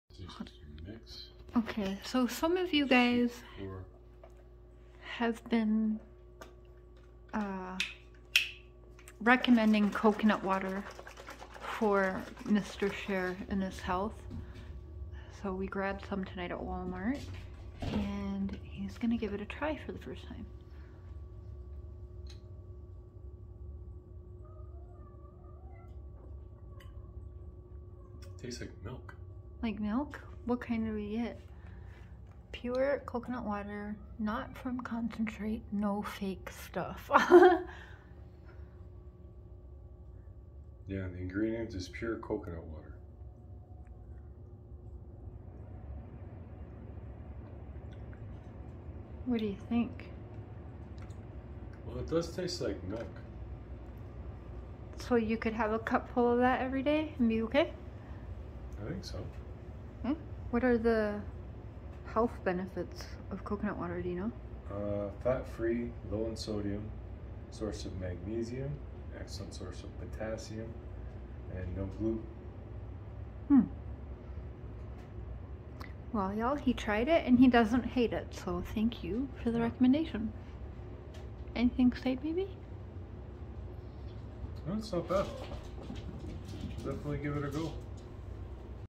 Trying your recommendations / Coconut Water 💧 😋
Trying some Coconut Water for the first time.
y'all have recommended that Mr. Try this.

we love all the advice y'all give.  thank you.
◇��◇ CHANNELS WE SUPPORT ◇☆◇

☆▪︎☆ WAYS TO TIP CHER ☆▪︎☆

☆▪︎☆ Cher's Amazon Wish List ☆

☆▪︎☆ Mr.Cher Amazon Wish List
Amazon Wish Lists: Prairie Boy77

$CrazyVoice